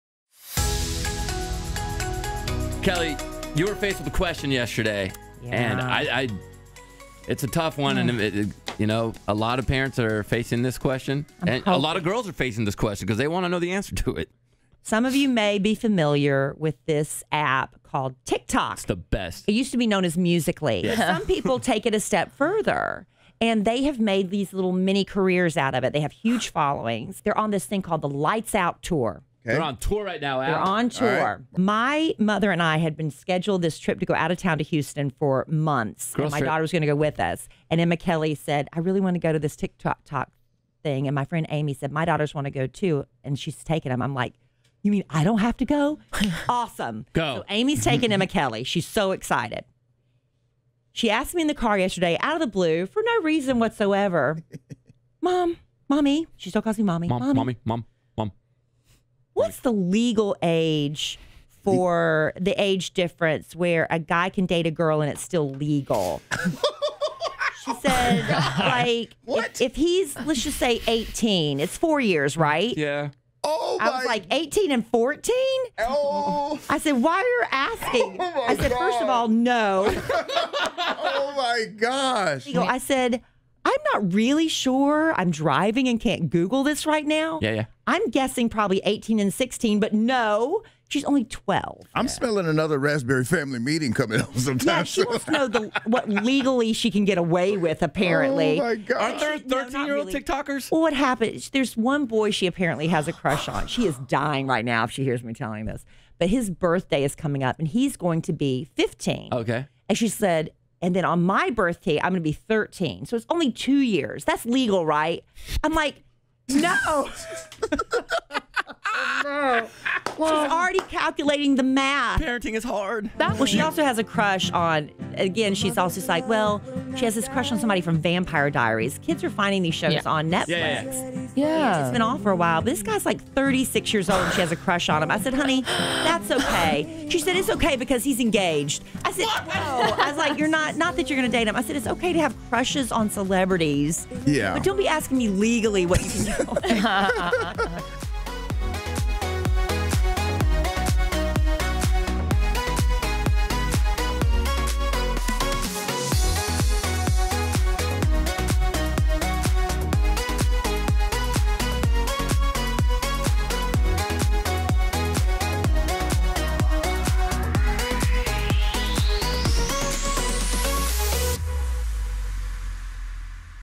Emma Kelly's Awkward Question
Emma Kelly asked her mom what?! What awkward questions have your kids asked you?